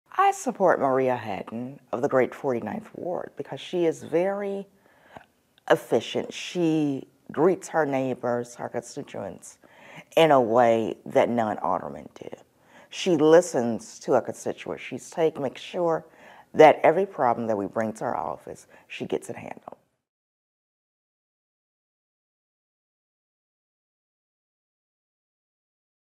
Re-elect Maria Hadden, Jocelyn's Testimonial
Maria Hadden deserves to be re-elected. Here are reasons why from the residents in the community.